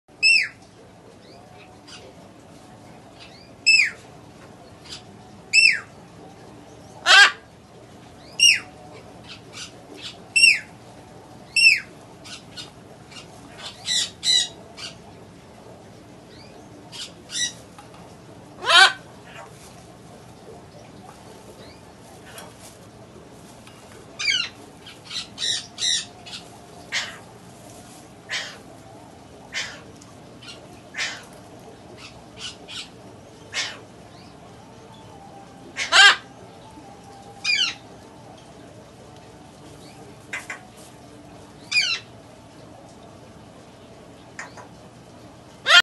BABY BUFFON MACAW SUPER TAME AVAILABLE BOTH LOCATION
Very cuddly tame doesn't bite at all very good talker make lovely pets. well known for taking ability and make lovley pet very good charecter Parrot. very healthy birds and there are on very good mixed diet